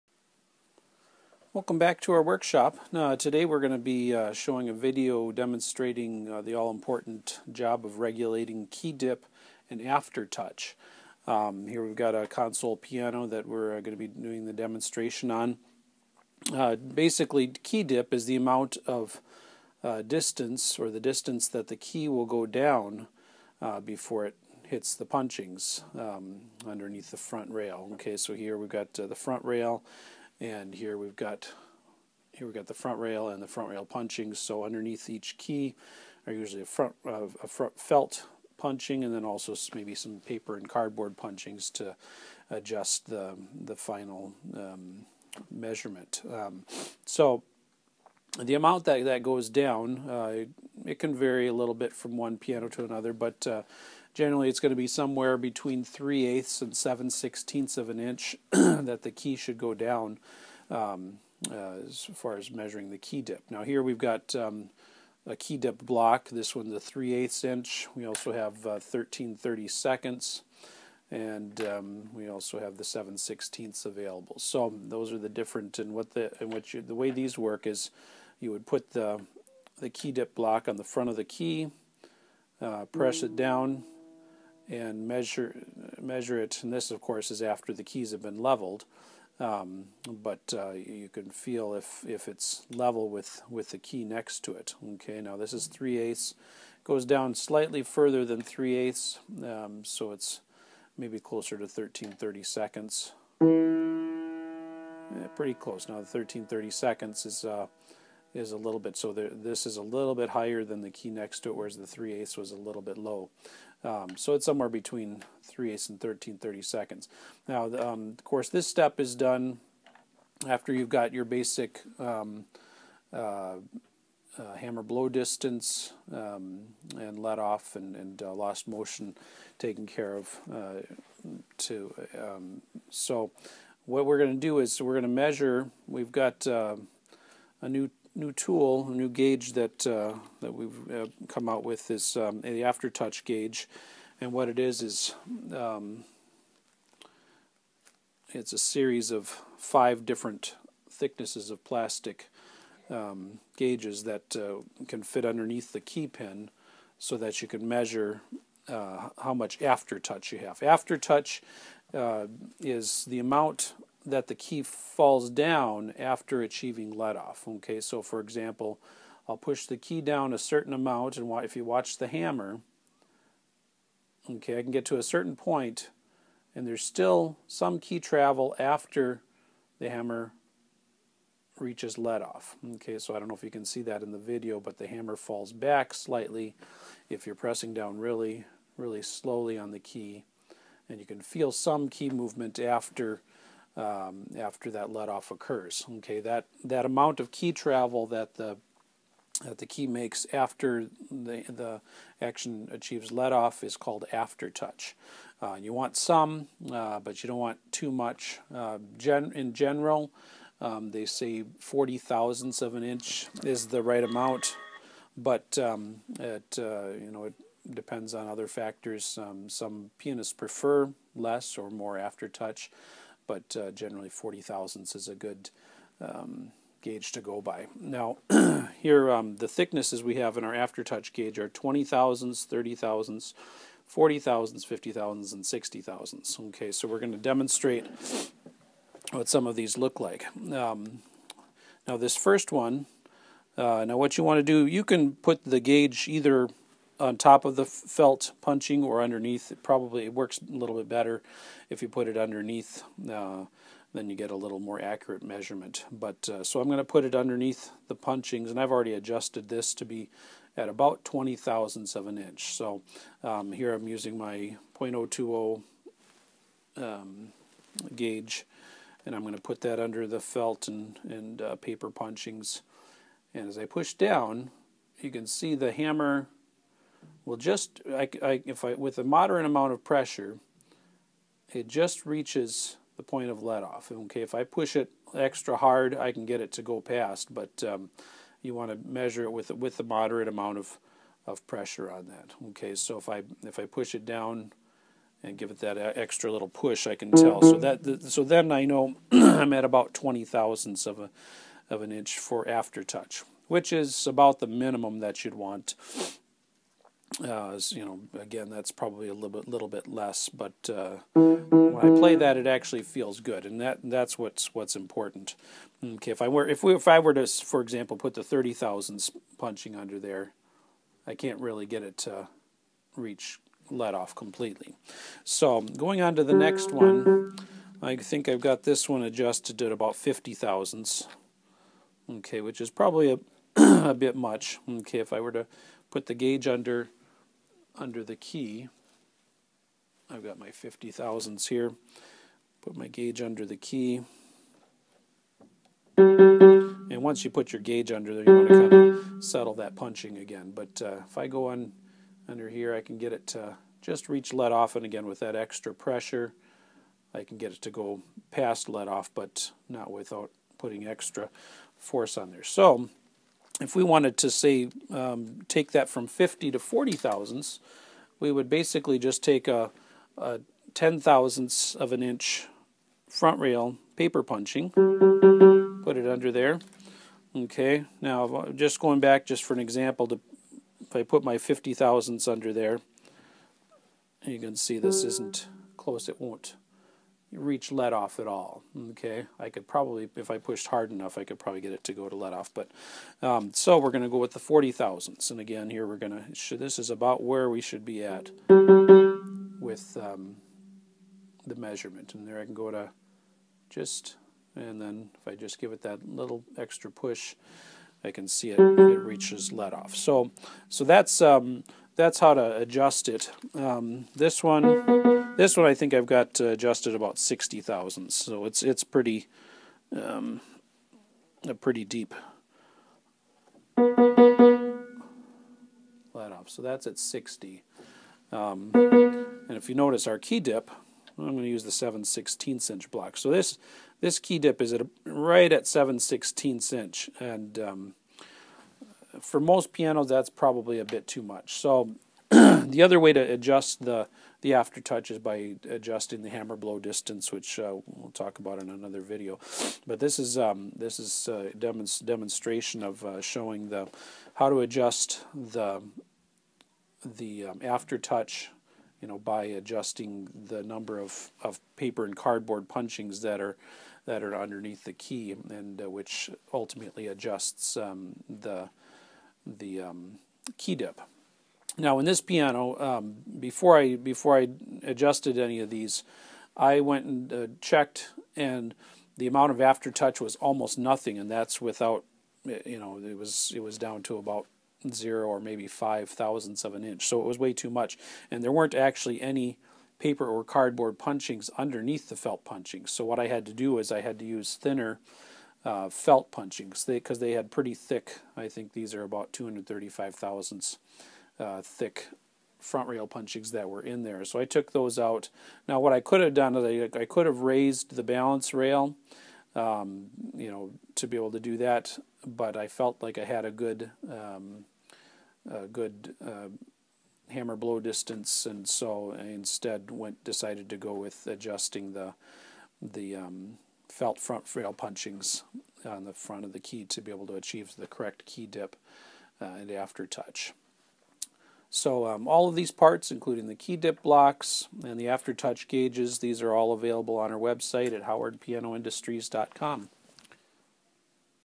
Regulating Key Dip and Aftertouch - Piano Tuning & Repair I HOWARD PIANO INDUSTRIES
One important step in regulating a piano action is to regulate the key dip and aftertouch. This video demonstrates the techniques and tools to use to accomplish this step in the piano regulation process.

🎥 Video Chapters
00:00 - Intro
01:03 - Key Dip Blocks
03:48 - Aftertouch Thickness
04:02 - Key Dip & Aftertouch Adjustment

☑️ Piano Parts, Tools, and Accessories